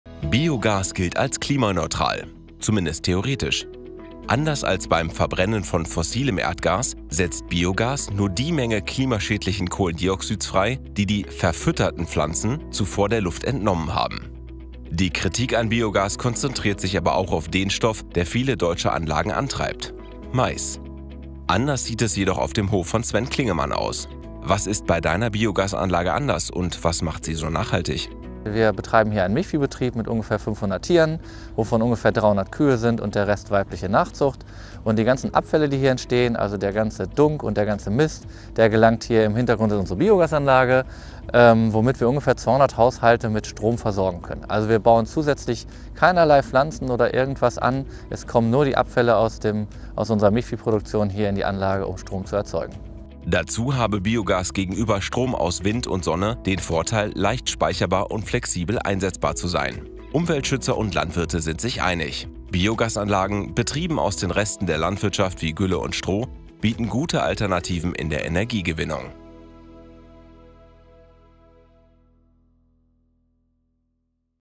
Biogas nur aus Gülle und Mist
Manche Biogasanlagen sind wahre Resteverwerter, besonders dann, wenn sie nur dem „gefüttert“ werden, was auf einem Milchbauernhof eh anfällt: Gülle und Mist. Neben seinem Betrieb kann Landwirt Sven Klingemann auch noch viele andere Haushalte nachhaltig mit Energie versorgen. Sollten eurer Meinung nach noch mehr solcher Anlagen gebaut werden?